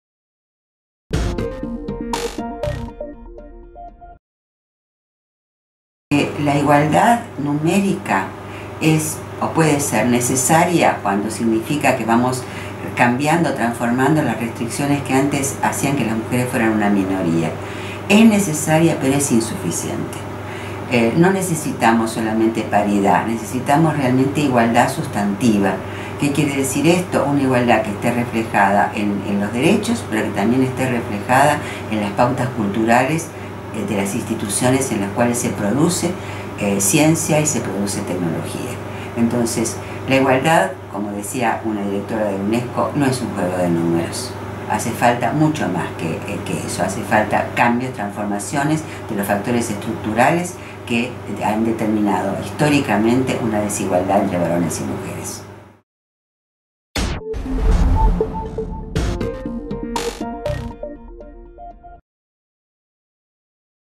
Gloria Bonder: igualdad de género
Gloria Bonder, Coordinadora de la Cátedra UNESCO Mujer, Ciencia y Tecnología en América Latina, y Directora del Área de Género, Sociedad y Políticas de FLACSO, Argentina, habla con SciDev.Net sobre la igualdad de género.